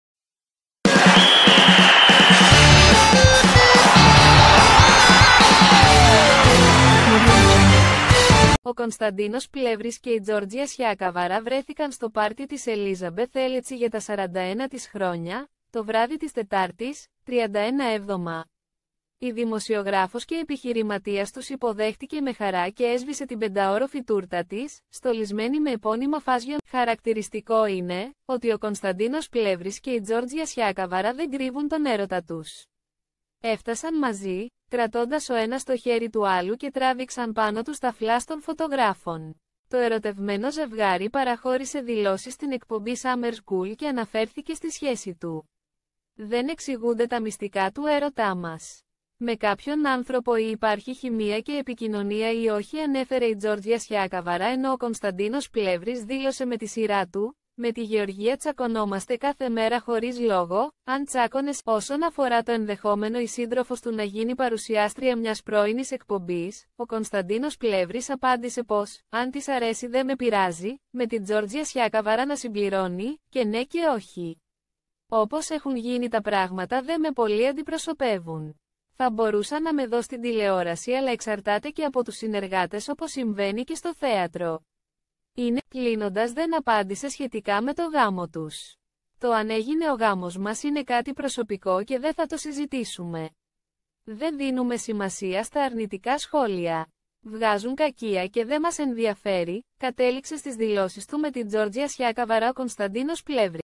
Κωνσταντίνος Πλεύρης Τζόρτζια Σιακαβάρα «Τσακωνόμαστε κάθε μέρα χωρίς λόγο»
Ο Κωνσταντίνος Πλεύρης και η Τζόρτζια Σιακαβάρα βρέθηκαν στο πάρτι της Ελίζαμπεθ Ελέτσι για τα 41 της χρόνια, το βράδυ της Τετάρτης (31/7). Η δημοσιογράφος και επιχειρηματίας τους υποδέχτηκε με χαρά και έσβησε την πενταώροφη τούρτα της, στολισμένη με επώνυμα fashion brands.